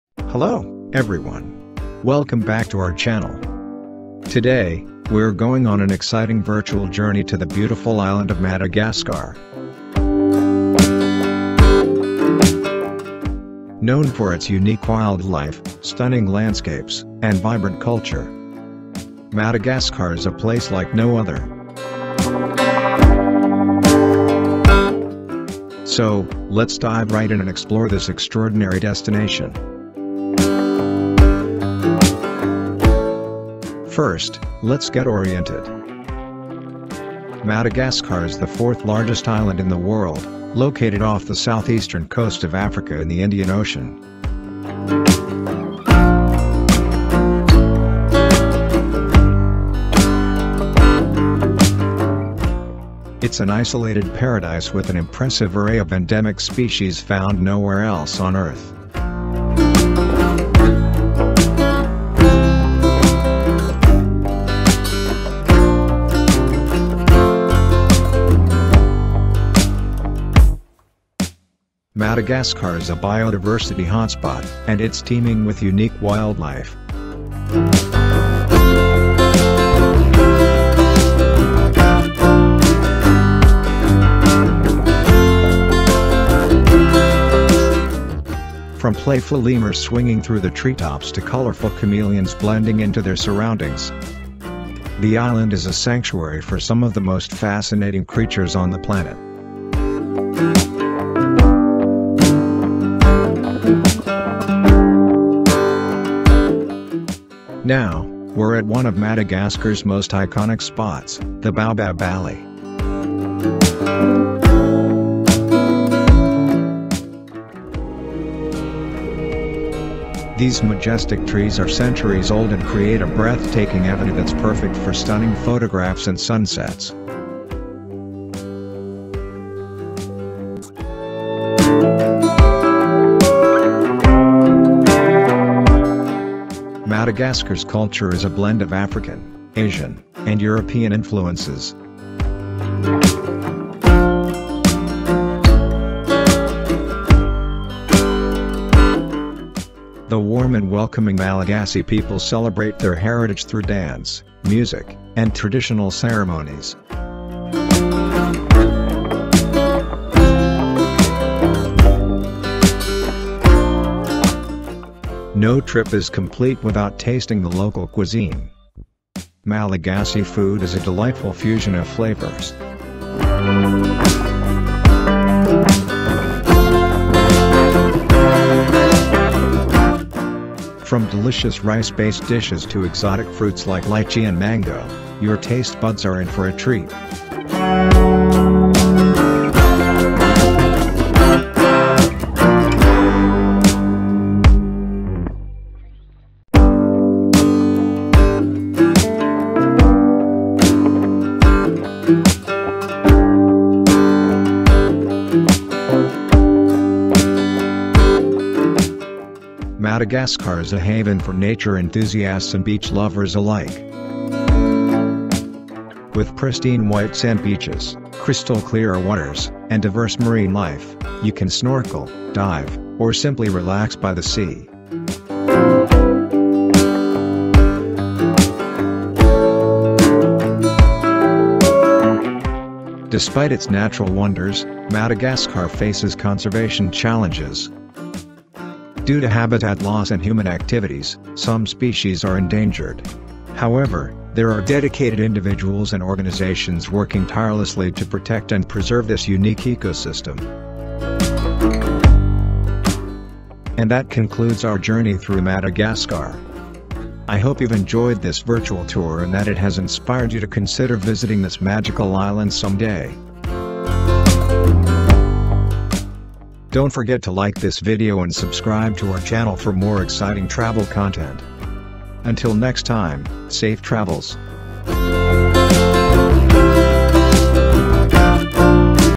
Unveiling the Hidden Wonders of Madagascar in 4k |Madagascar Country Full Documentary
Hello, everyone! Today,  we're going on an exciting virtual journey to the beautiful island of Madagascar. Known for its unique wildlife, stunning landscapes, and vibrant culture, Madagascar is a place like no other. So, let's dive right in and explore this extraordinary destination!
First, let's get oriented. Madagascar is the fourth largest island in the world, located off the southeastern coast of Africa in the Indian Ocean. It's an isolated paradise with an impressive array of endemic species found nowhere else on Earth.
Madagascar is a biodiversity hotspot, and it's teeming with unique wildlife. From playful lemurs swinging through the treetops to colorful chameleons blending into their surroundings, the island is a sanctuary for some of the most fascinating creatures on the planet.
Now, we're at one of Madagascar's most iconic spots - the Baobab Alley. These majestic trees are centuries old and create a breathtaking avenue that's perfect for stunning photographs and sunsets.
Madagascar's culture is a blend of African, Asian, and European influences. The warm and welcoming Malagasy people celebrate their heritage through dance, music, and traditional ceremonies. No trip is complete without tasting the local cuisine. Malagasy food is a delightful fusion of flavors. From delicious rice-based dishes to exotic fruits like lychee and mango, your taste buds are in for a treat!
Madagascar is a haven for nature enthusiasts and beach lovers alike. With pristine white-sand beaches, crystal-clear waters, and diverse marine life, you can snorkel, dive, or simply relax by the sea.
Despite its natural wonders, Madagascar faces conservation challenges. Due to habitat loss and human activities, some species are endangered. However, there are dedicated individuals and organizations working tirelessly to protect and preserve this unique ecosystem.I hope you've enjoyed this virtual tour and that it has inspired you to consider visiting this magical island someday. Until next time, safe travels!